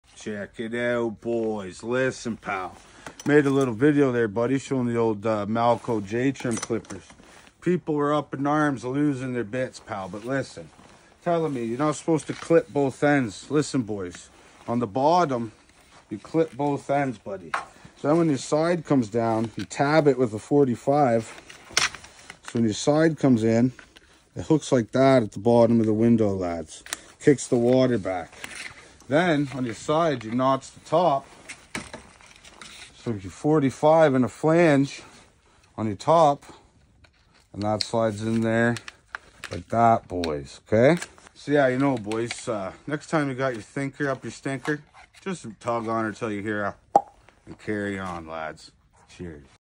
How to J channel
check it out boys! here's a video to clear up a few questions about how to j channel and about the malco j trim clippers! yes the bottom gets notched on both ends, the sides get a tab & 45 at the bottom and a clip at the top, the top gets 45 and flanger on each end so it can fold down and fasten! CHEERS